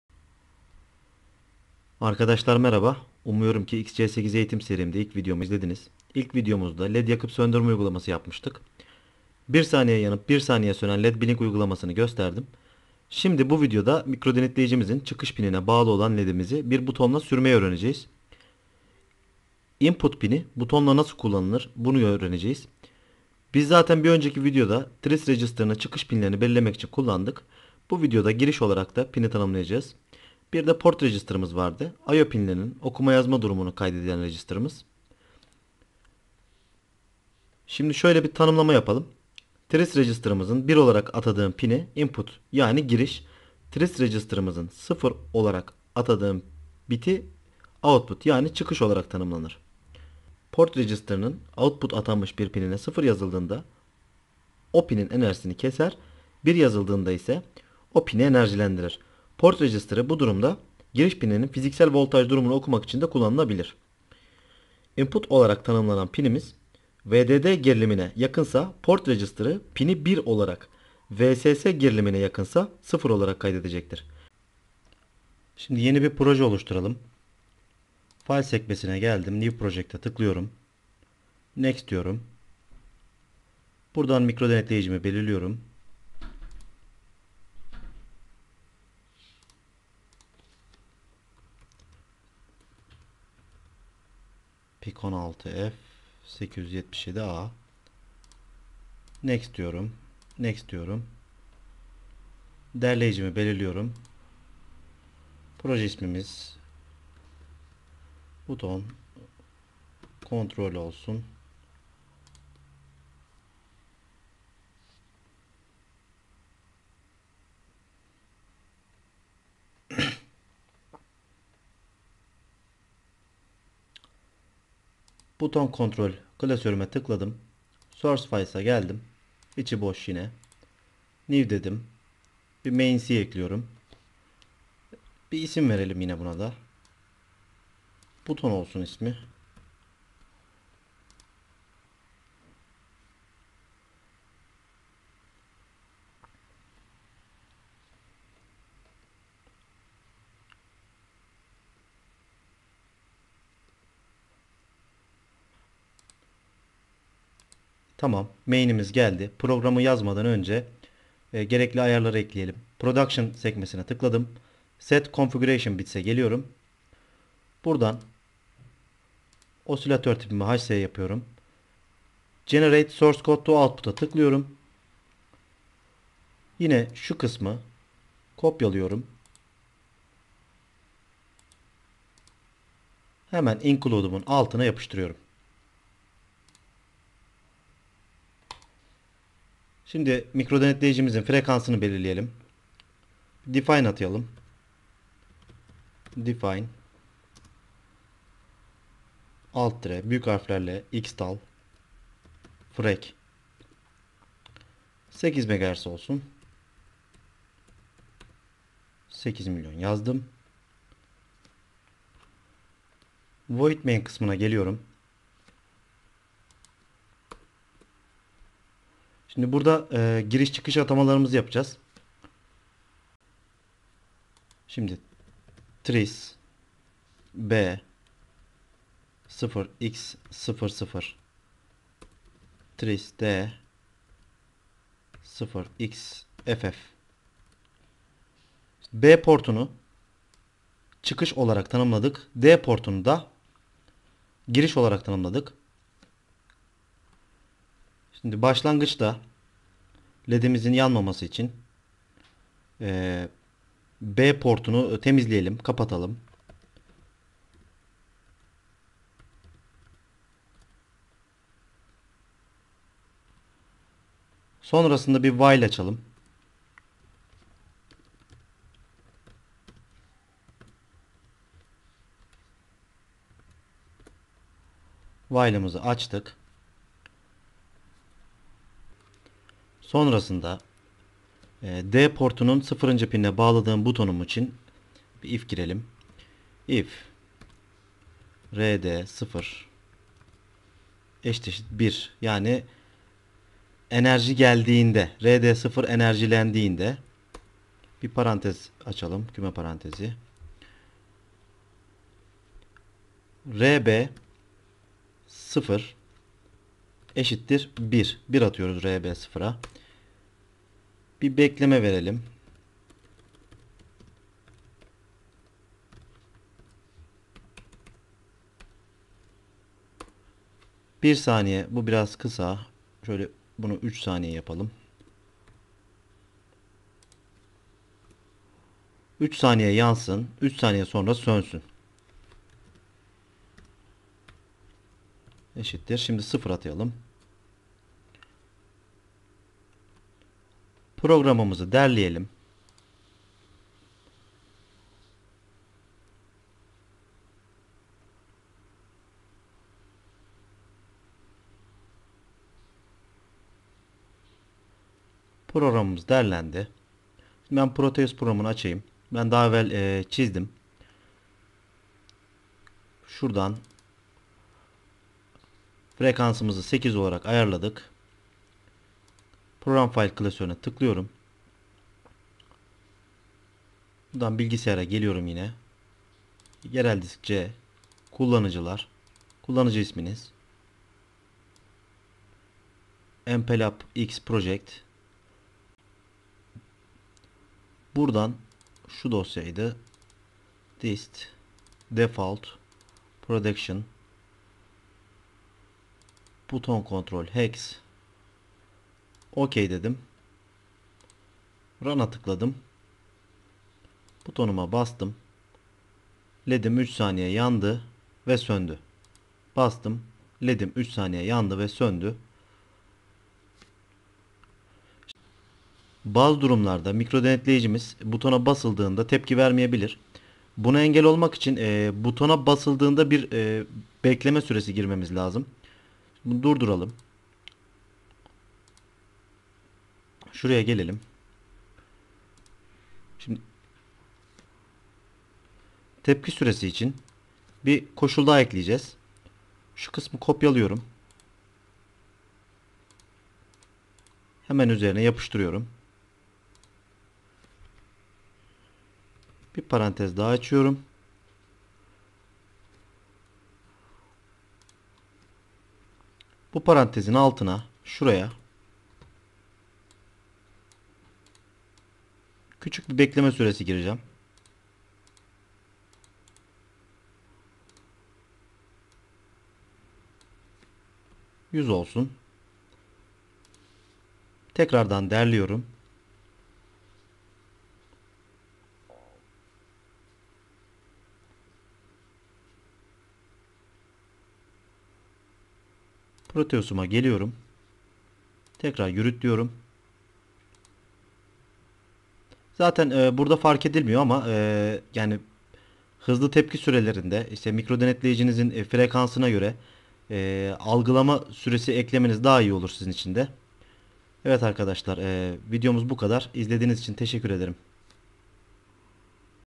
MPLAB XC8 PIC16F877A Butonla LED Sürme | BUTTON CONTROL AND LED DRIVING
Pull Down bağlı bir butonla Led Sürme.
BUTTON CONTROL AND LED DRIVING